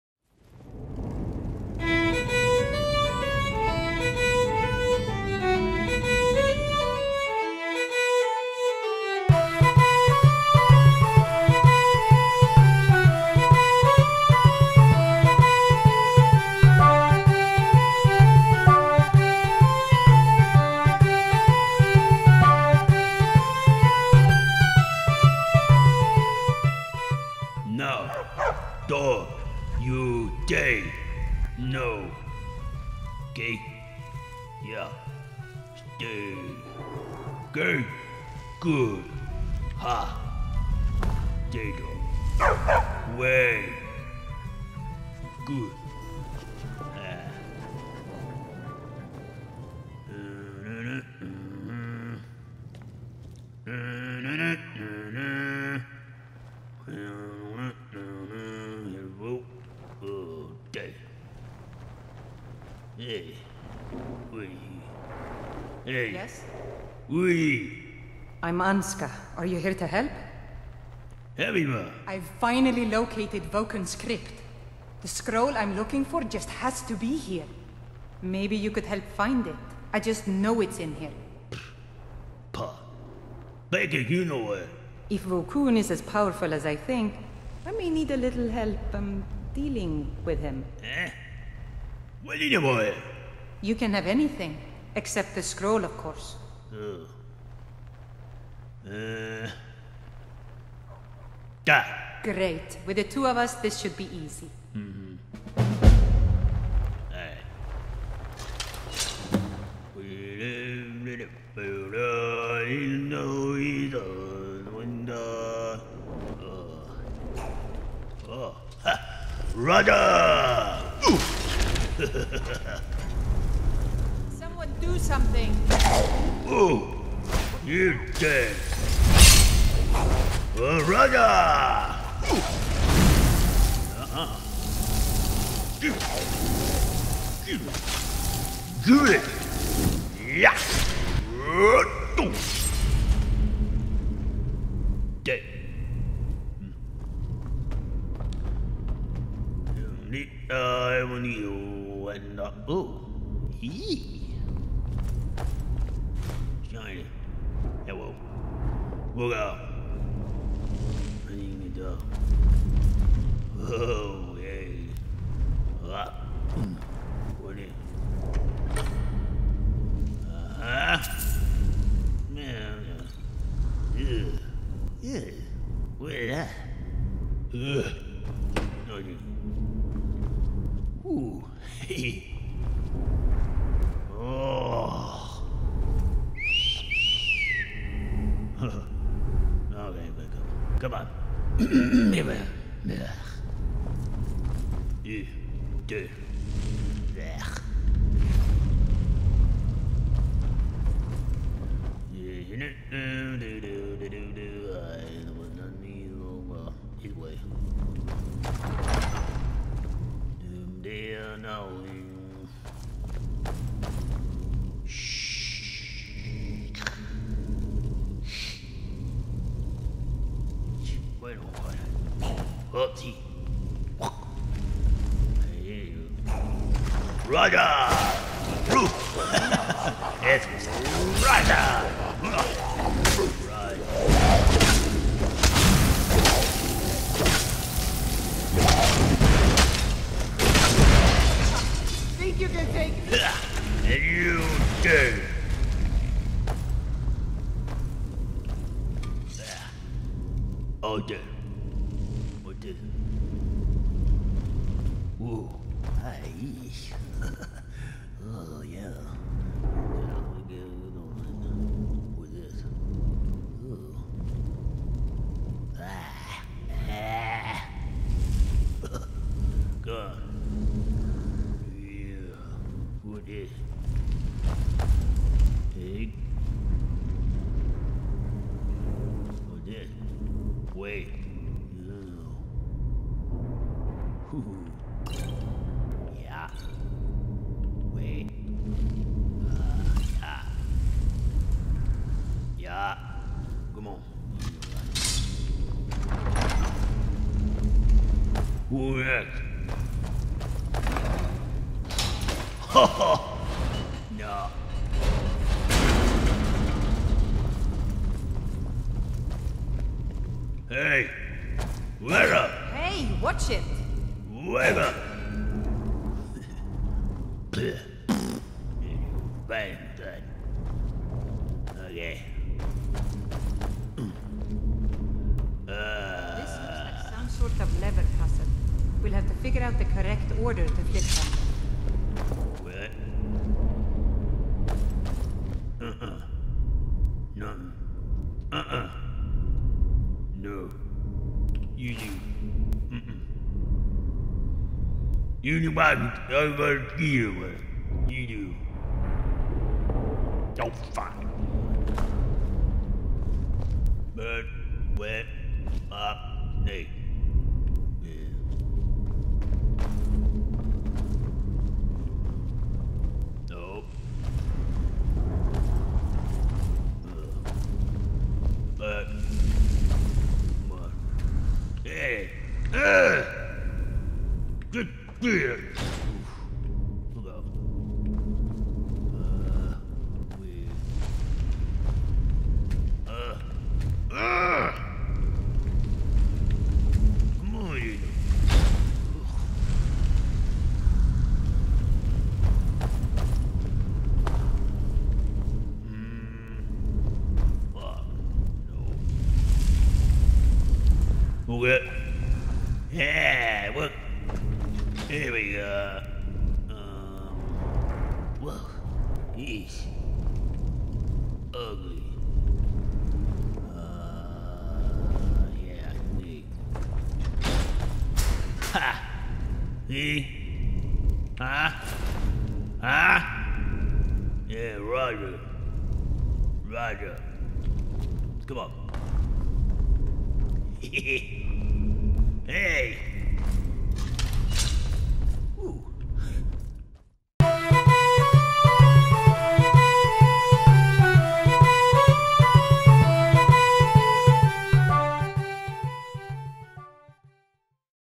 Season 1: ROGER! Hates Vokun - Part 1
DRAUGR LAGER PRESENTS
An Undead Theatre Production

A Micro Adventure
Staring: Roger the Draugr

"I Never Liked Vokun Anyway!"

ROGER! Hates Vokun - Part 1

Roger and Dog travel to High Gate Ruin

NOTE:
Do not ask for a mod list, none will be provided